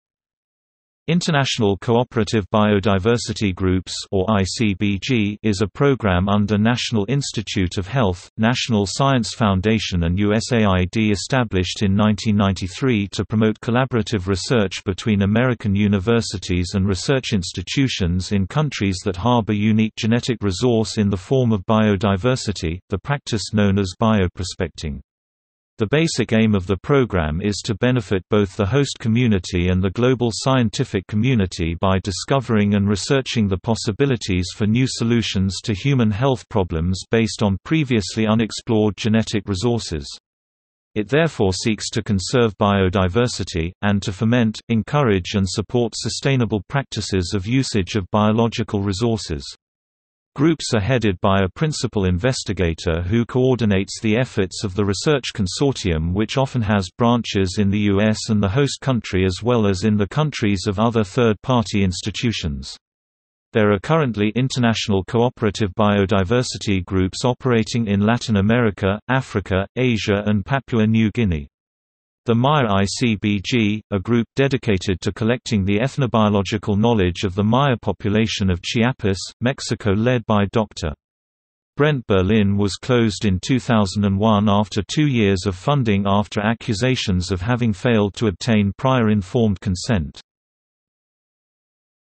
International Cooperative Biodiversity Group | Wikipedia audio article
- Socrates

SUMMARY
International Cooperative Biodiversity Groups (or ICBG) is a program under National Institute of Health, National Science Foundation and USAID established in 1993 to promote collaborative research between American universities and research institutions in countries that harbor unique genetic resource in the form of biodiversity - the practice known as bioprospecting. The basic aim of the program is to benefit both the host community and the global scientific community by discovering and researching the possibilities for new solutions to human health problems based on previously unexplored genetic resources. It therefore seeks to conserve biodiversity, and to foment, encourage and support sustainable practices of usage of biological resources. Groups are headed by a principal investigator who coordinates the efforts of the research consortium which often has branches in the US and the host country as well as in the countries of other third party institutions. There are currently International Cooperative Biodiversity groups operating in Latin America, Africa, Asia and Papua-New Guinea. The Maya ICBG, a group dedicated to collecting the ethnobiological knowledge of the Maya population of Chiapas, Mexico led by Dr. Brent Berlin was closed in 2001 after two years of funding after accusations of having failed to obtain prior informed consent.